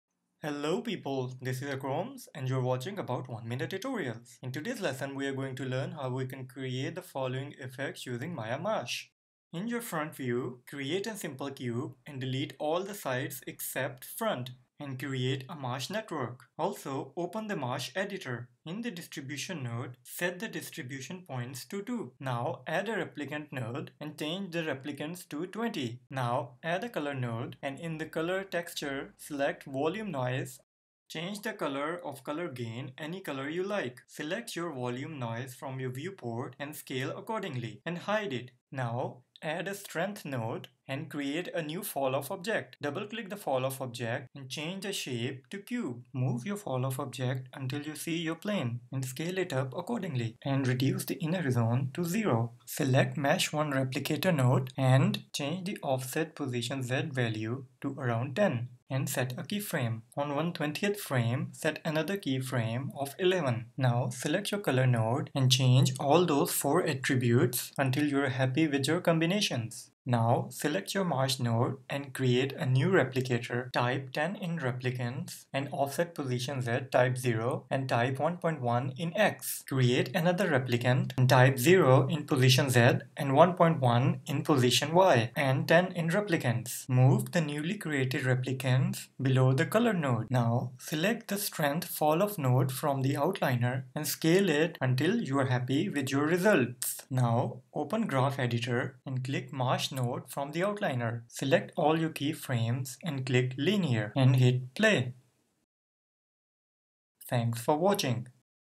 Motion Graphics Series 002 Hypno-Tiles Maya 2017
In todays lesson we are going to learn how to animate planes using maya mash plugin.